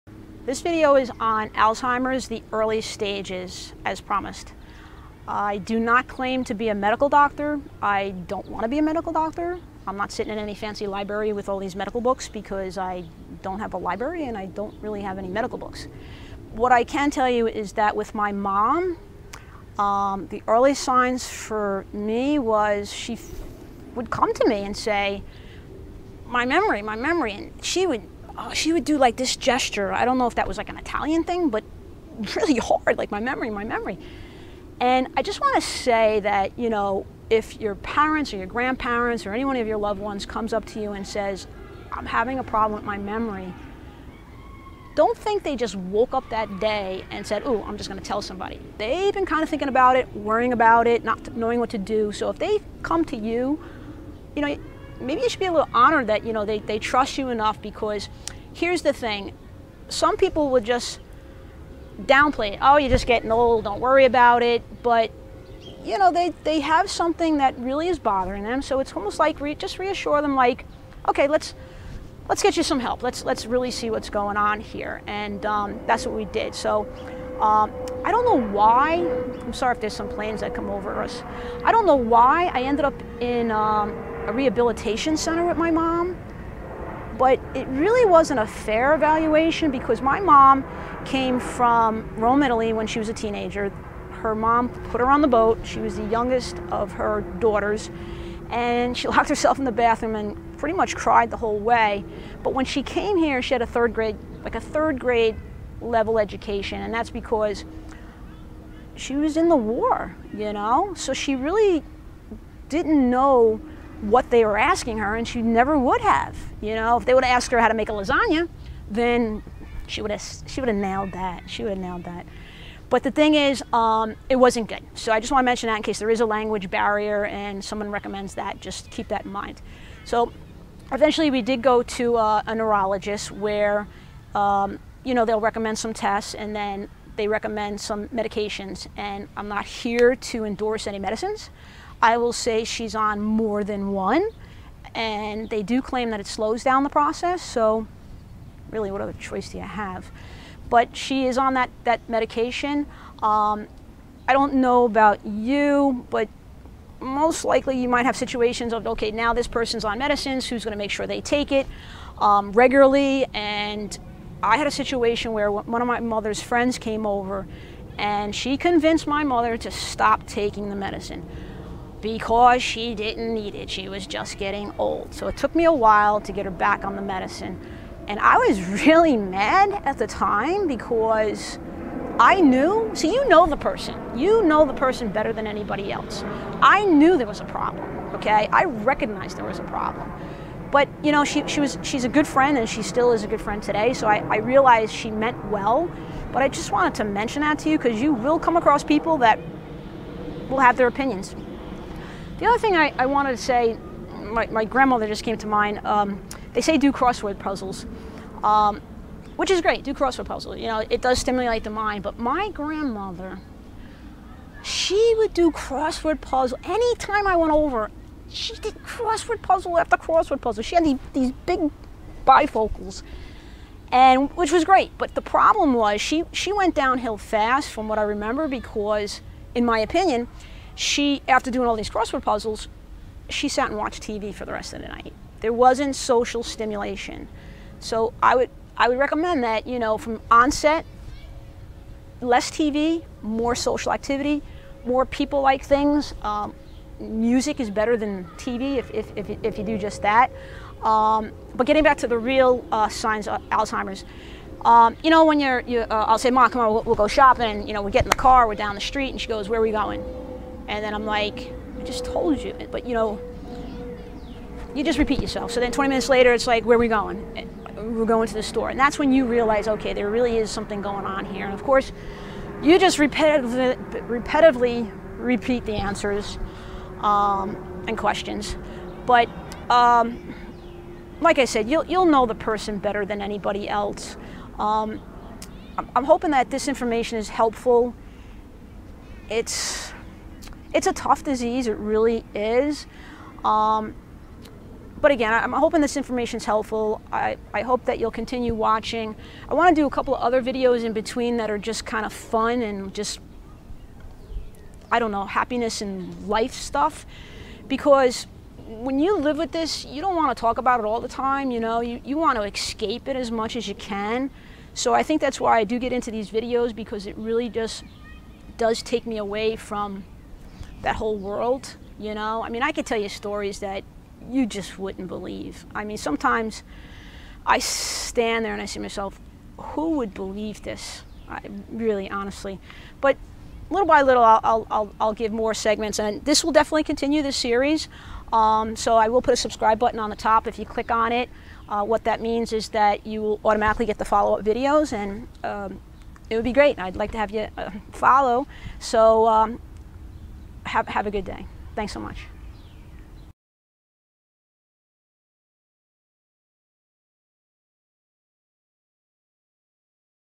Alzheimer's Early Signs.... Please Share
Alzheimer's early signs. Here I'll explain the very beginning signs for my mother as her Alzheimer's came on so unannounced. I hope this is helpful.